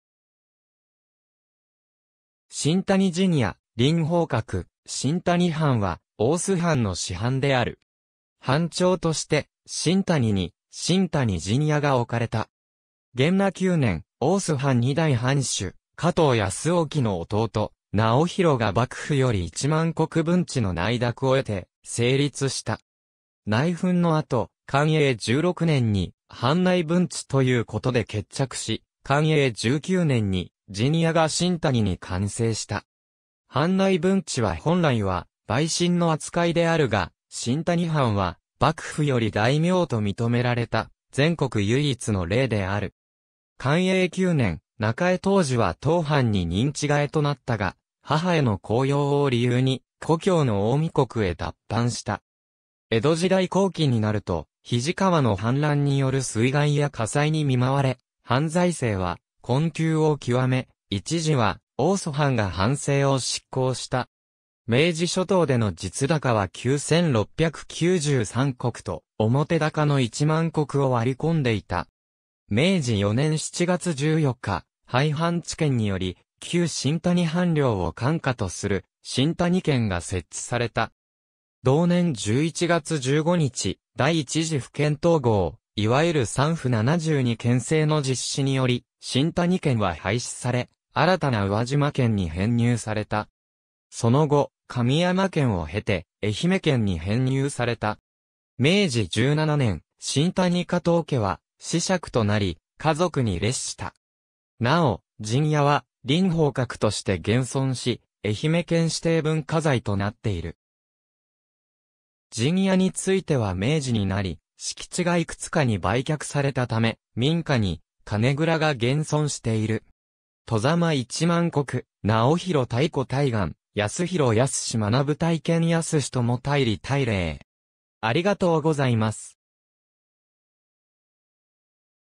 新谷藩
新谷陣屋「麟鳳閣」 新谷藩（にいやはん）は、大洲藩の支藩である。
藩庁として新谷（現・大洲市内）に新谷陣屋が置かれた。
元和9年（1623年）、大洲藩2代藩主・加藤泰興の弟・直泰が幕府より1万石分知の内諾を得て成立した。
内紛の後、寛永16年（1639年）に藩内分知ということで決着し、寛永19年（1642年）に陣屋が新谷に完成した。
藩内分知は本来は陪臣の扱いであるが、新谷藩は幕府より大名と認められた全国唯一の例である。
寛永9年（1632年）、中江藤樹は当藩に任地替えとなったが、母への孝養を理由に故郷の近江国へ脱藩した。
江戸時代後期になると、肱川の氾濫による水害や火災に見まわれ、藩財政は困窮を極め、一時は大洲藩が藩政を執行した。
明治初頭での実高は9,693石と、表高の1万石を割り込んでいた。
明治4年（1871年）7月14日、廃藩置県により、旧新谷藩領を管下とする新谷県（草高1万石、現石4890石）が設置された。
同年11月15日、第1次府県統合、いわゆる3府72県制の実施により新谷県は廃止され、新たな宇和島県に編入された（本庁・宇和島、支庁・大洲）。
その後、神山県を経て愛媛県に編入された。
明治17年（1884年）、新谷加藤家は子爵となり、華族に列した。
なお、陣屋は麟鳳閣として現存し、愛媛県指定文化財となっている。
陣屋については明治になり、敷地がいくつかに売却されたため、民家に金蔵が現存している（他に大坂、松本城などに現存）。
外様　1万石　（1623年 - 1871年） 直泰 泰觚 泰貫 泰広 泰宦 泰賢 泰儔 泰理 泰令